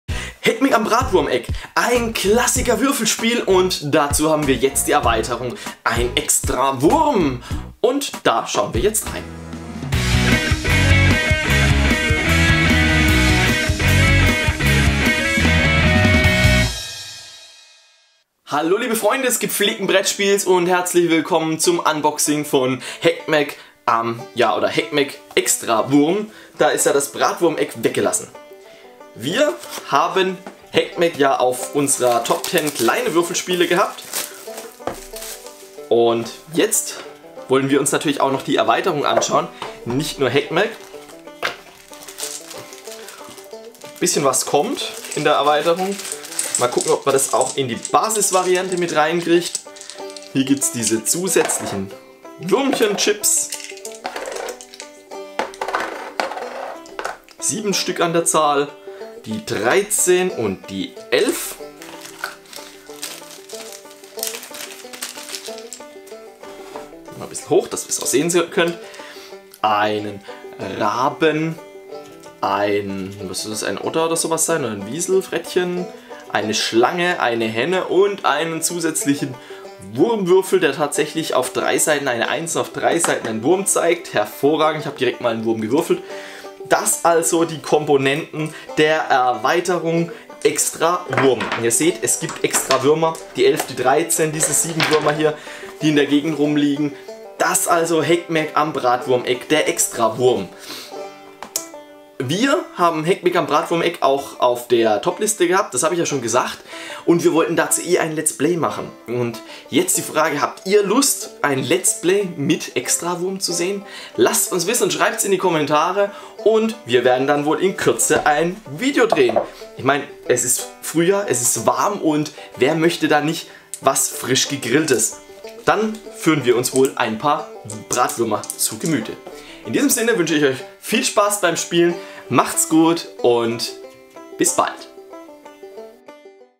Unboxing - Heckmeck am Bratwurmeck - Extrawurm (Zoch 2015)
Ein weiteres Unboxing, dieses mal "Heckmeck Extrawurm"
Hierzu wird auch in kürze eine Regelerklärung und ein Let's Play kommen. Schaut auf unserer Homepage bei den Exklusiven Videos vorbei!